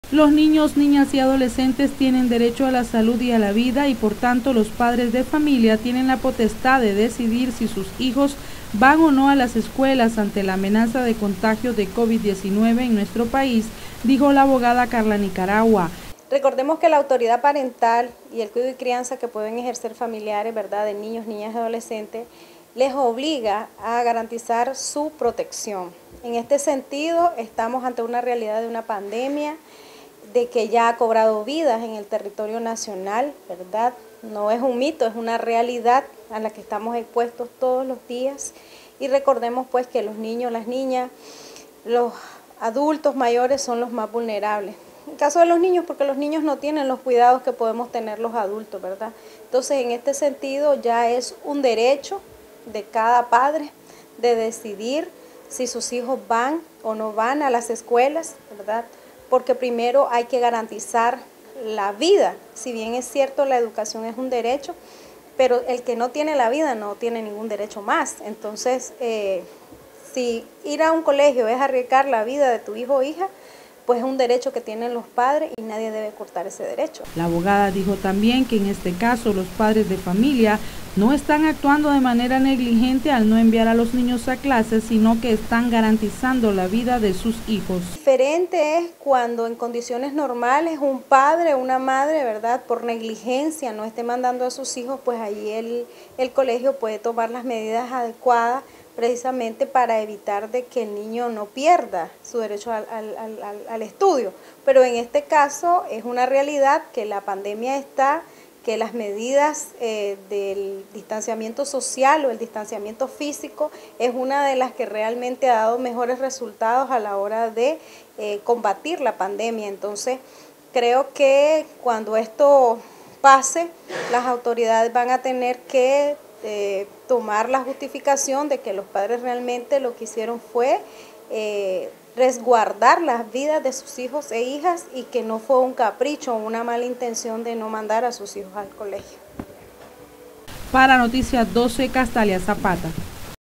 Padres de familia tienen derecho a no mandar a sus hijos a clases ante el COVID-19
La abogada Karla Nicaragua, aseguró que los padres de familia tienen el derecho a no mandar a sus hijos a clases ante el Coronavirus.